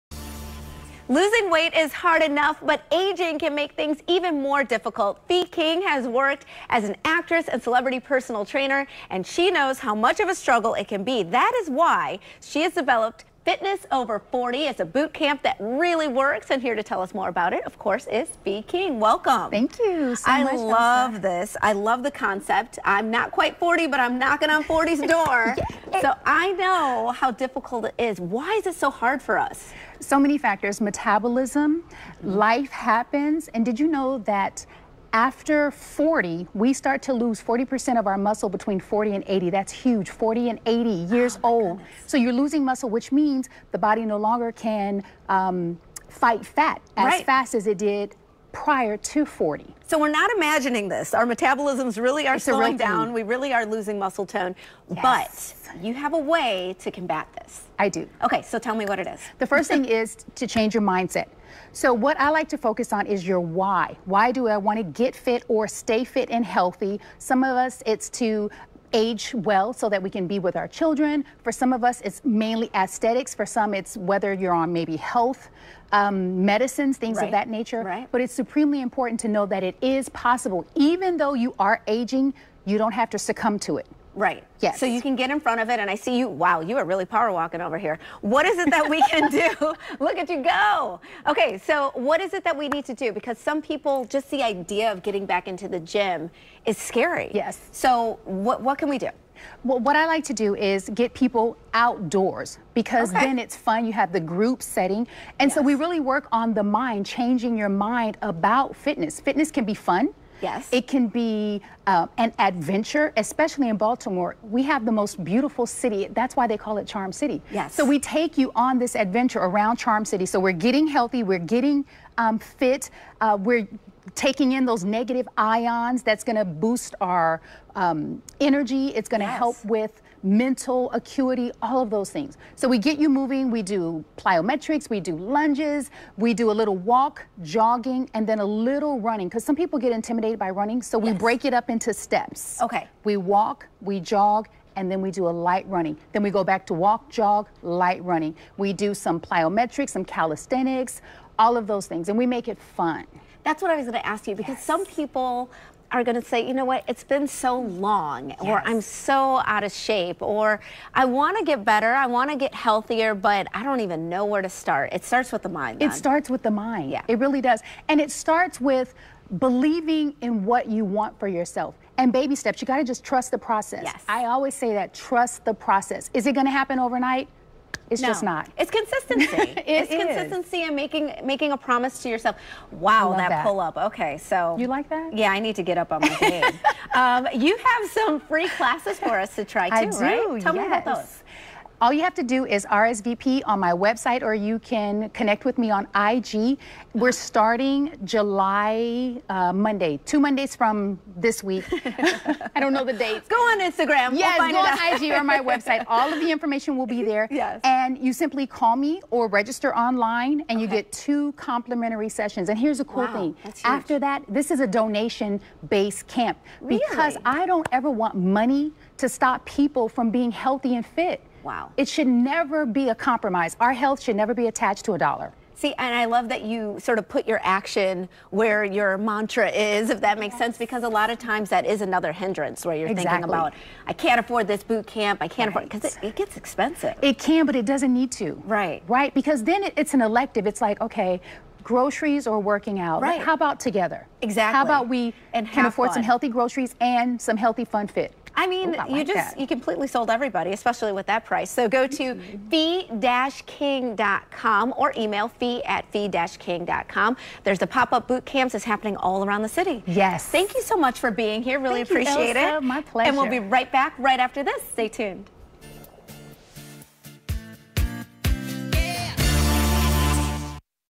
FitnessOver40- UpLyft Baltimore: WMAR Midday Maryland Interview
Time to stop running from life's Challenges, Obstacles, Roadblocks and Setbacks and use Fee King's crawl before you walk, jog, run and soar in your life 'Moving Meditation' as a way to connect with self while burning fat, lessening stress, sweating out and detoxifying the body, tightening, toning and firming those hard to re-train OVER 40 body parts.

We meet every Monday @6AM at Druid Hill Park - Baltimore City

My Mott is:  If NotNOW; When